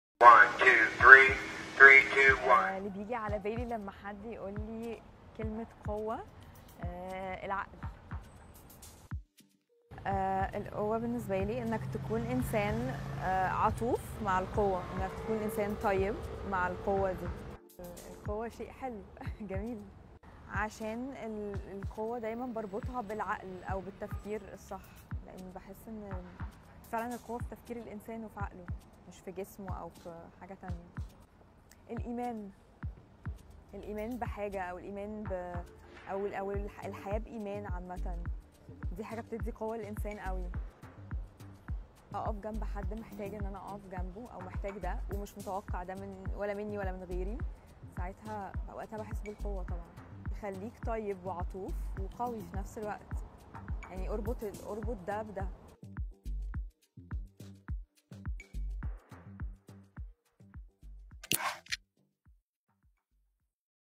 Haya Magazine x Bulgari ft. Huda El Mufti 'What Gives You Strength?' Film by Natacha Mantovani
Film: Natacha Mantovani
Photo: Jeremy Zaessinger
MUA: Alisson Fetouaki
Hair: Laura Chadwick
Styling: Sarah Cazeneuve